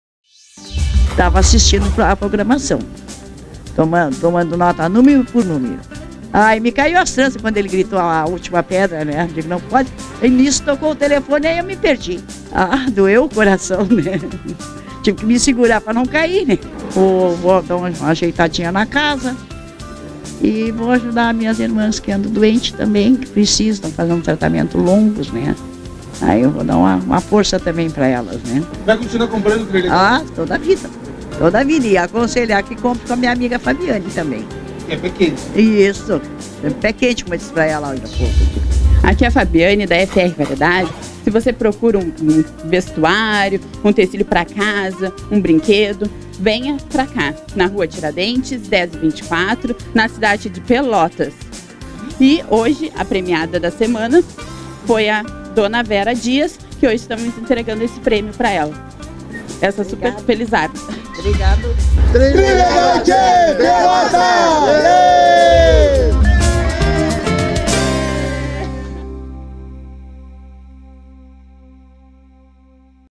Matéria Ganhadora 2o sorteio - 02.09.18
Ganhadora de R$ 50 mil:
Vera Dias de Pelotas.

Certificado de Contribuição somente para o interior do Rio Grande do Sul - Brasil.